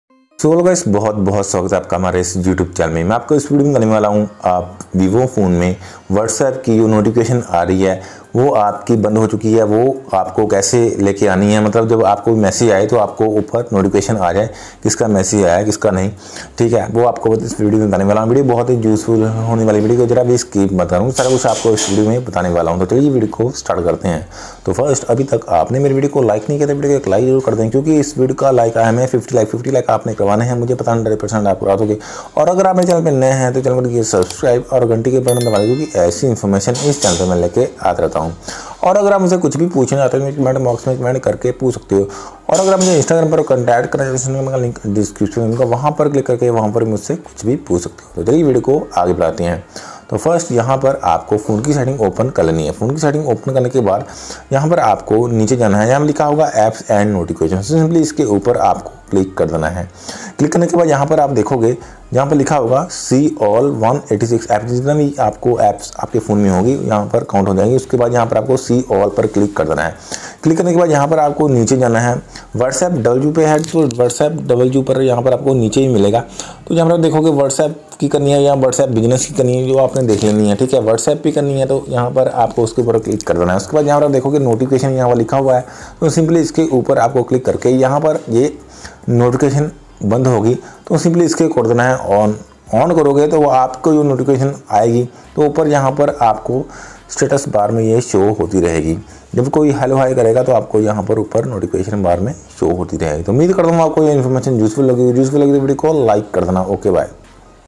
How to Fix Whatsapp Notification Problem in VIVO Y20 And Y20i // VIVO Y20 Me Whatsapp Notification p
How to Fix Whatsapp Notification Problem In VIVO Y20 And Y20i // Vivo Y20 Me Whatsapp Notification Problem Kaise Solve Kare

Note:  all pitchers show in the video belonges to the respected owners and not me ..I am not the owner of any picher show in the video